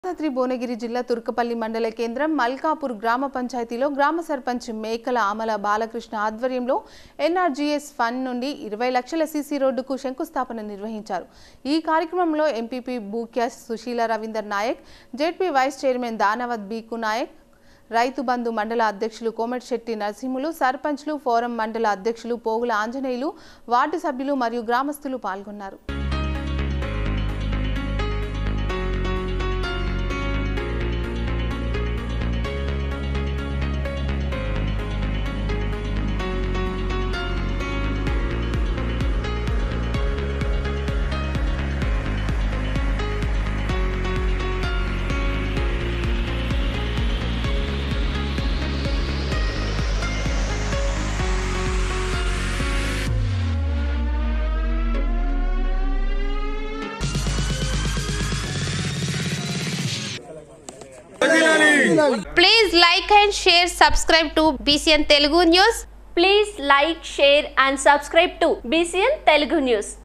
సీసీ రోడ్డుకు శంకుస్థాపన || Bhuvanagiri
.యాదాద్రి భువనగిరి జిల్లా,
తుర్కపల్లి మండల కేంద్రం, మల్కాపూర్ గ్రామపంచాయతీ లొ గ్రామ సర్పంచ్ మేకల అమల బాలకృష్ణ  ఆధ్వర్యంలో  NRGS ఫండ్ నుండి  ఇరవై లక్షల సీసీ రోడ్డుకు శంకుస్థాపన నిర్వహించారు  .ఈ కార్యక్రమం లొ ఎంపీపీ భూక్యా సుశీల రవీందర్ నాయక్, జడ్పీ వైస్ చేర్మెన్ దానవత్ భికూ నాయక్,  రైతు బందు మండల అధ్యక్షులు కోమటిశెట్టి నర్సింలు ,సర్పంచ్ ల ఫోరమ్ మండల  అధ్యక్షులు  పోగుల ఆంజనేయులు, వార్డు సభ్యులు మరియు గ్రామస్తులు పాల్గొన్నారు